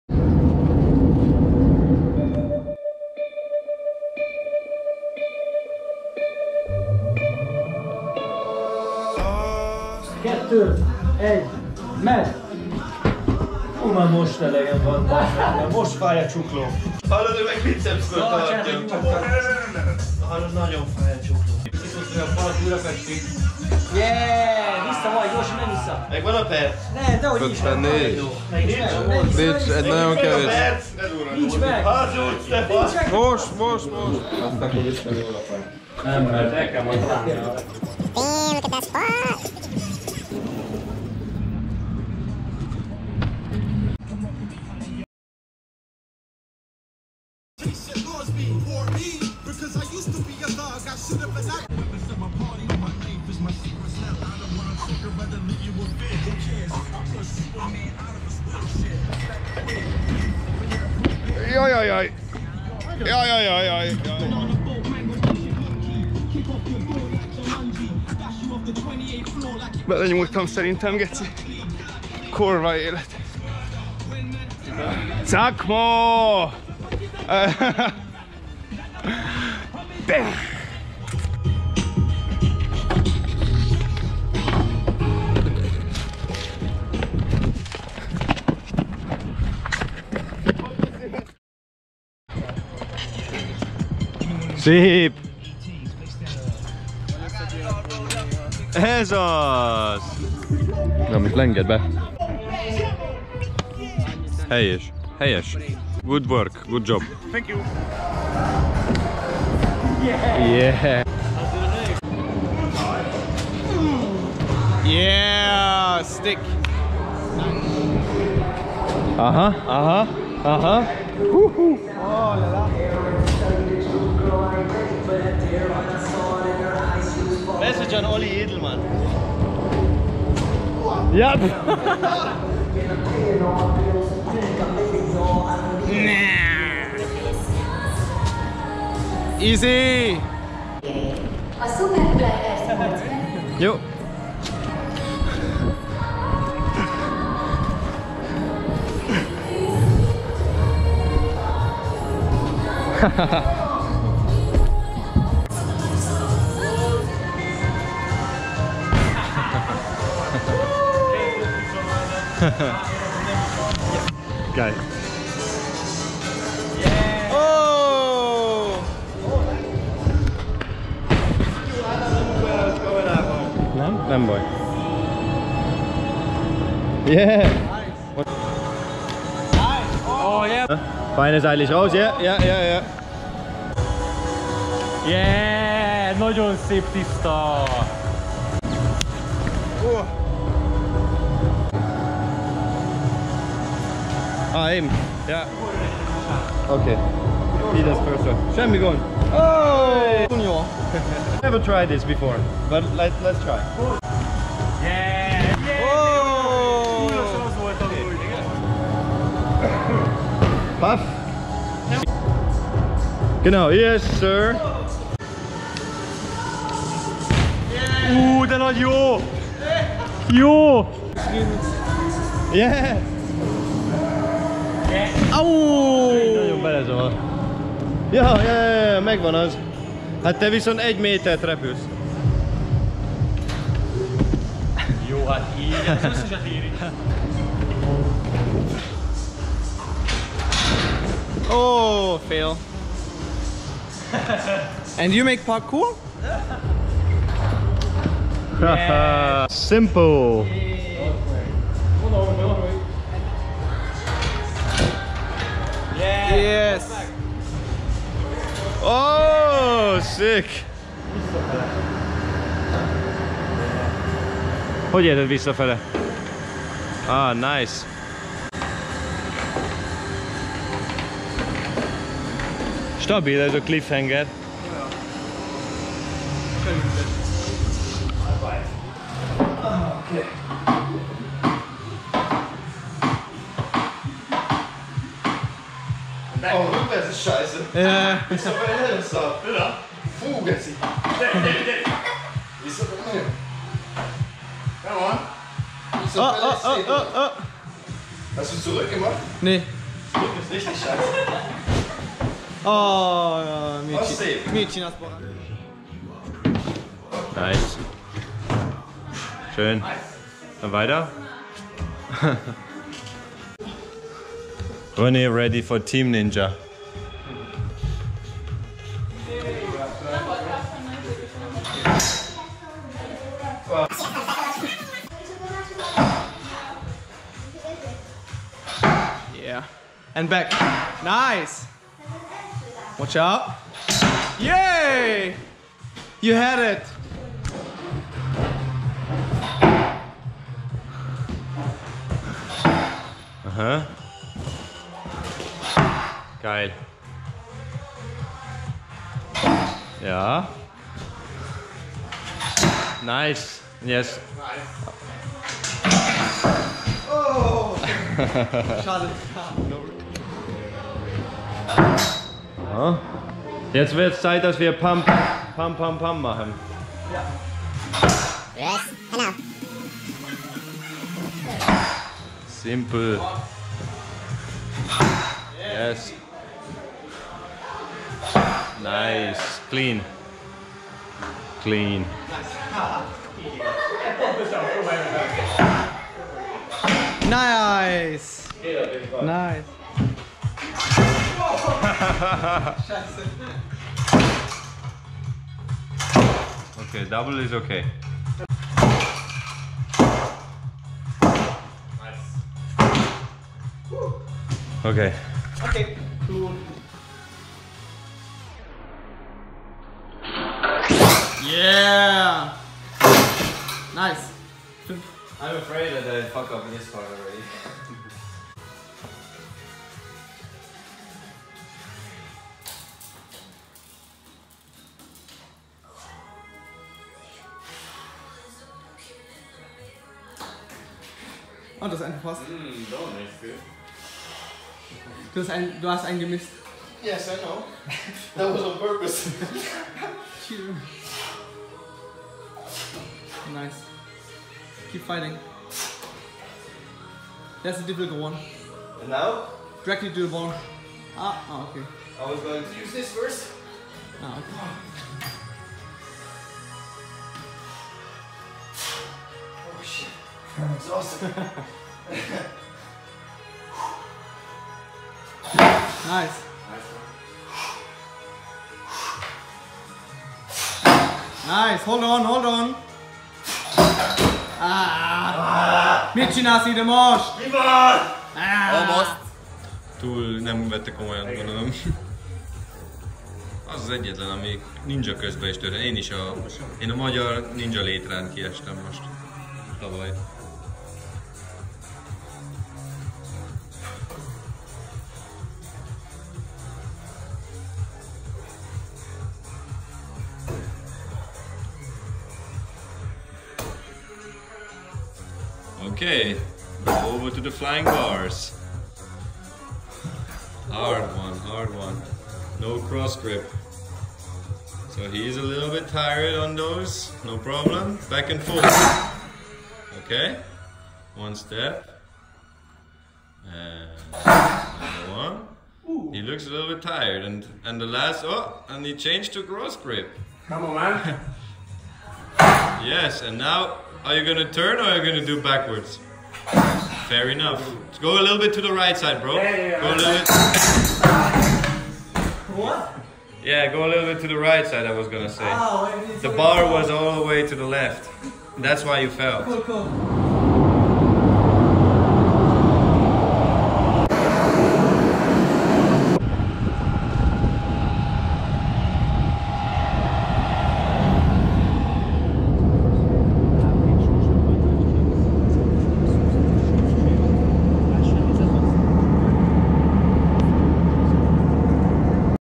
Just a Quicky in Budapest - HarmaTv #18
I was 3 days off for training & to visit some good friends in Hungary!

I had the Honor to train with the Master Rene Cassely & Buszàs Gàbor st the „Ninja Barlang“

This is a simple edit just to remember the good times, didnt feel like that was enough for a full vlog, so i did this.